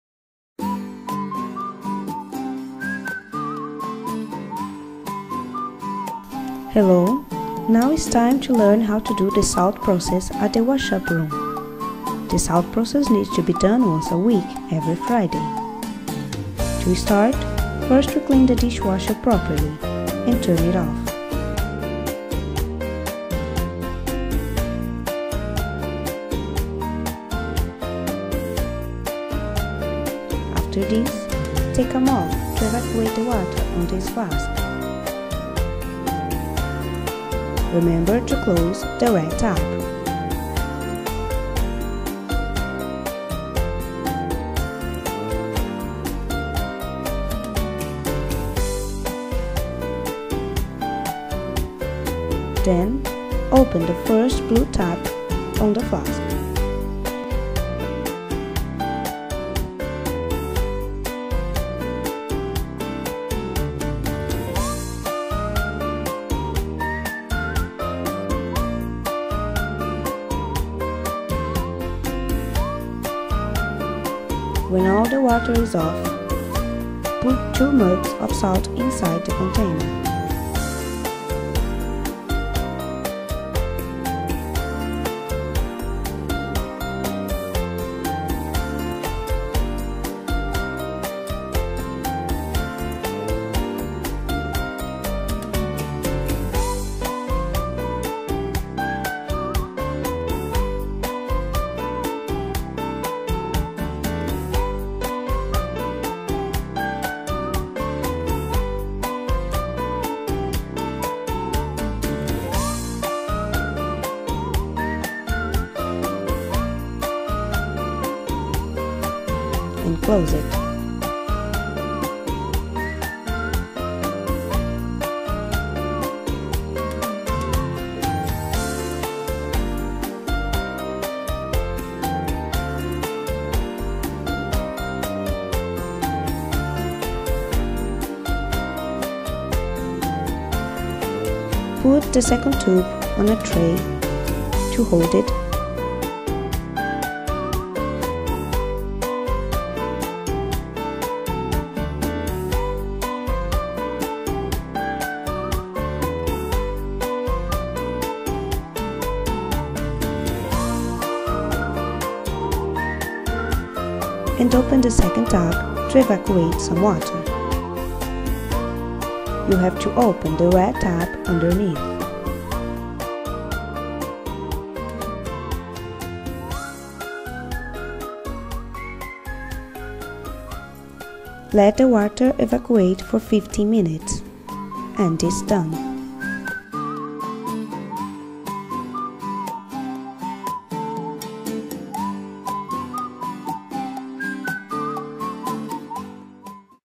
Dishwasher twice weekly salt process
Training video how to do salt process properly at St. Rhadagunds